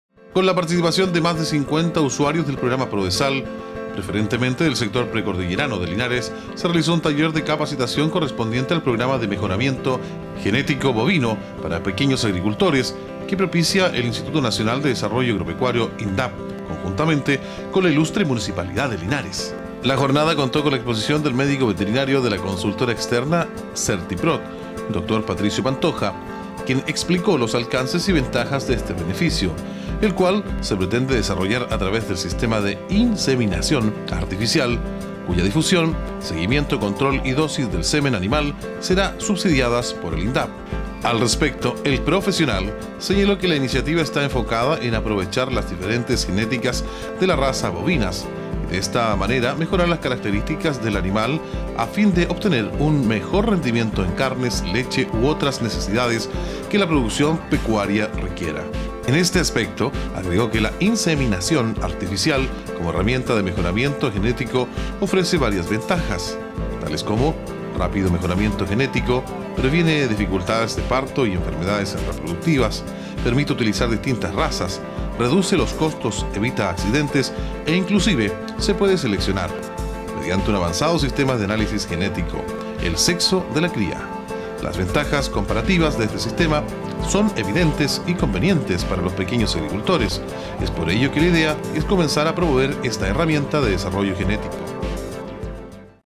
Usuarios Prodesal de Linares se capacitan en mejoramiento Genético Bovino
Con la participación de más de 50 usuarios del Programa Prodesal, preferentemente de sectores precordilleranos se realizó un taller de capacitación correspondiente al Programa de Mejoramiento Genético Bovino para pequeños agricultores, que propicia el Instituto Nacional de Desarrollo Agropecuario (Indap), conjuntamente con la Municipalidad de Linares.

La jornada contó con la exposición del Médico Veterinario de la consultora externa Sertiprod Doctor Patricio Pantoja, quien explicó los alcances y ventajas de este beneficio, el cual se pretende desarrollar a través del sistema de inseminación artificial, cuya difusión, seguimiento, control y dosis de semen animal serán subsidiadas por el Indap.